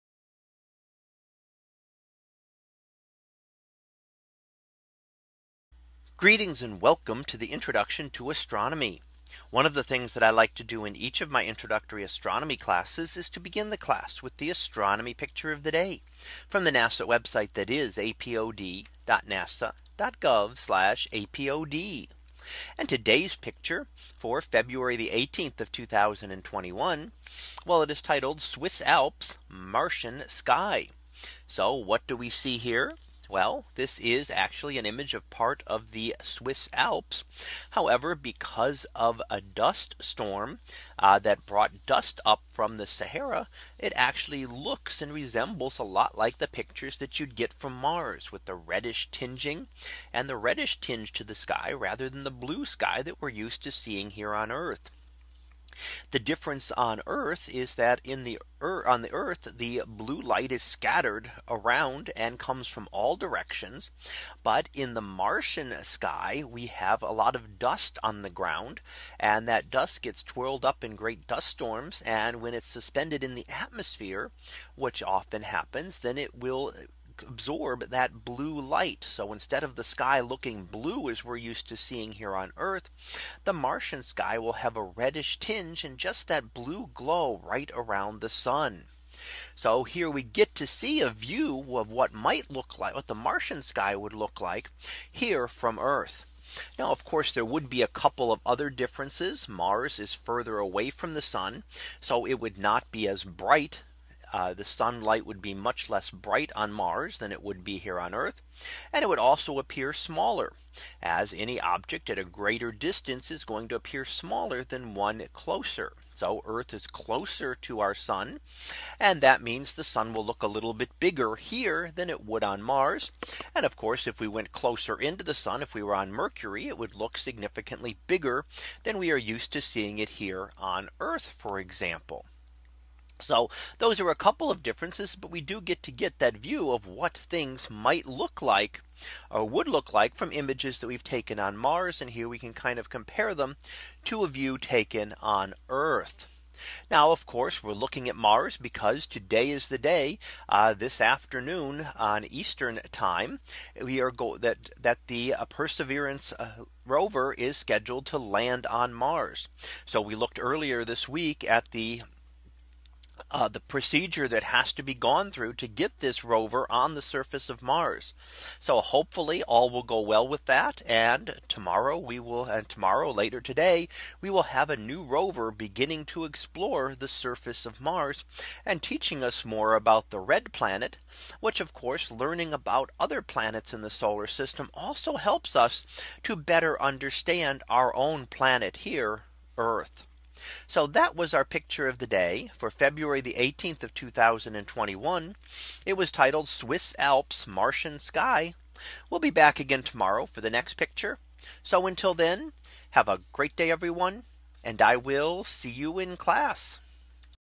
2021 February 18 - Swiss Alps, Martian Sky
In today's image, we see a view of the Swiss Alps which resembles the sky of Mars. This is from dust from the Sahara Desert that absorbs the blue light of the sky giving it the reddish color we see from images of Mars.